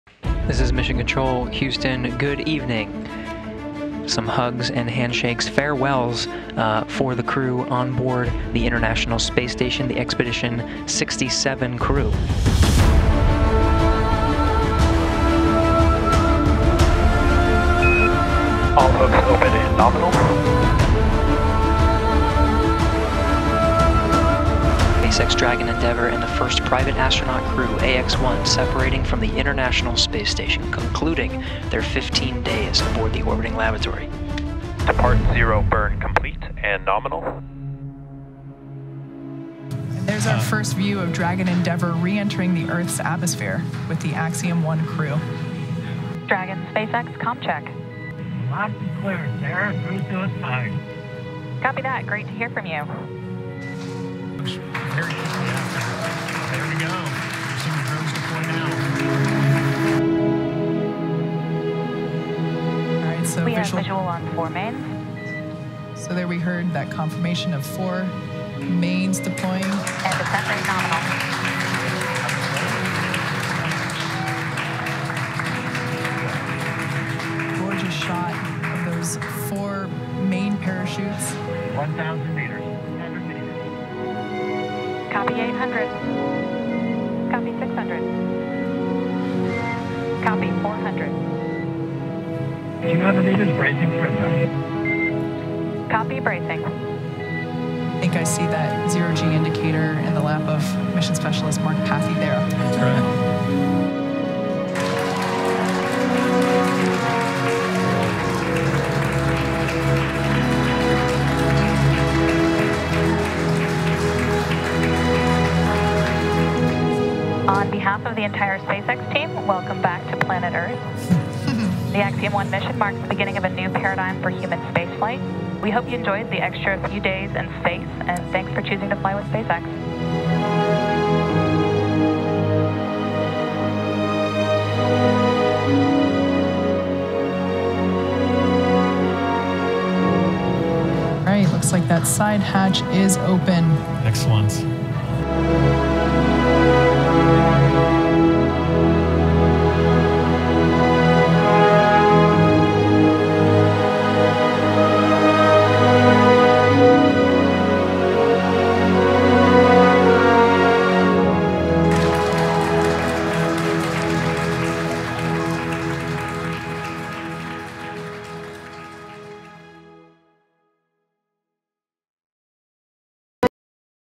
משדר הנחיתה - הרגעים הגדולים
אחרי 17 יום בחלל, האסטרונאוט הישראלי איתן סטיבה נחת בשלום בכדור הארץ.
אילו הרגעים הגדולים מהנחיתה: ההתנתקות מתחנת החלל, הכניסה לאטמוספירה, פתיחת המצנחים ולבסוף הנחיתה בים - הספלאשדאון והיציאה מהחללית